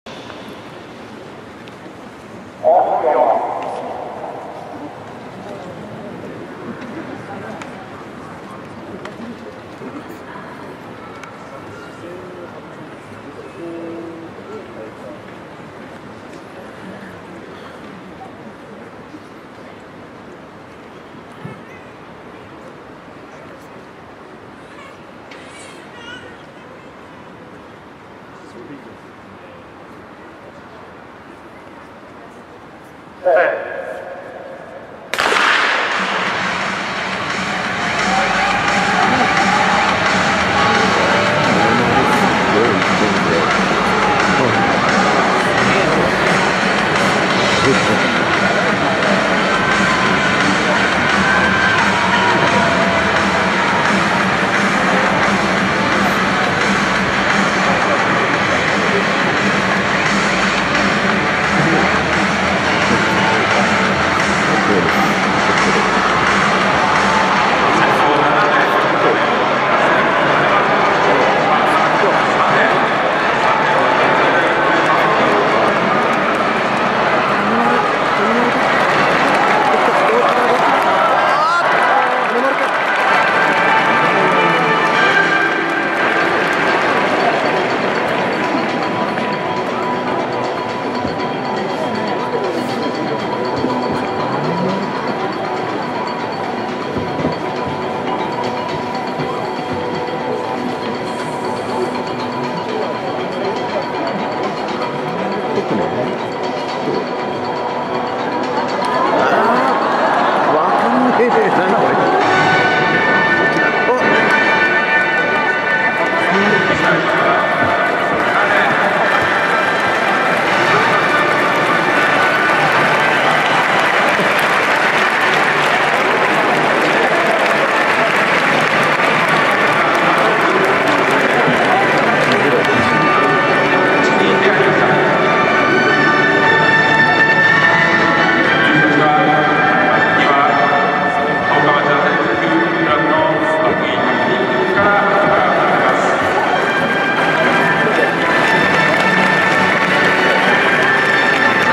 日本陸上2015男子400m決勝 金丸祐三 46.10
2015年6月28日 新潟 デンカ ビックスワン スタジアム